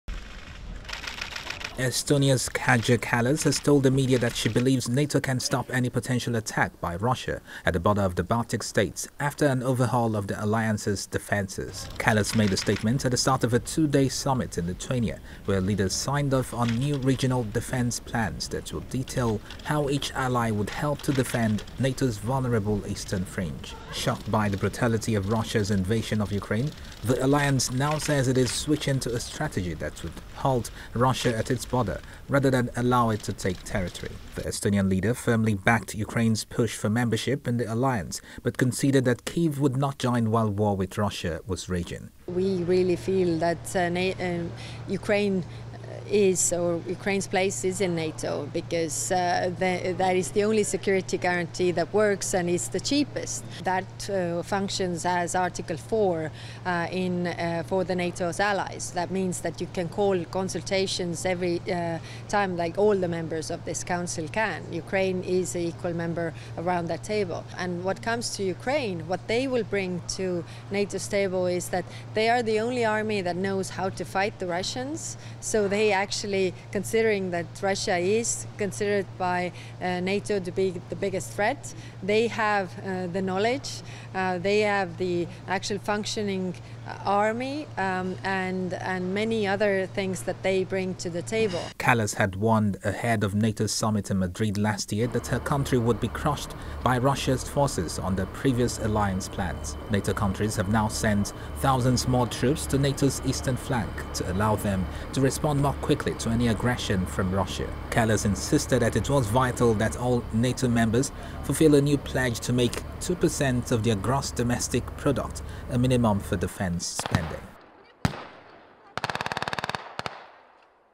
ESTONIA CALLS FOR READINESS TO STOP POTENTIAL RUSSIA INVASION
Estonia's Kaja Kallas has told the media that she believes NATO can stop any potential attack by Russia at the border of the Baltic states after an overhaul of the alliance's defences.
Kallas made the statement at the start of a two-day summit in Lithuania where leaders signed off on new regional defence plans that will detail how each ally would help to defend NATO's vulnerable eastern fringe.
Shocked by the brutality of Russia's invasion of Ukraine, the alliance now says it is switching to a strategy that would halt Russia at its border rather than allow it to take territory.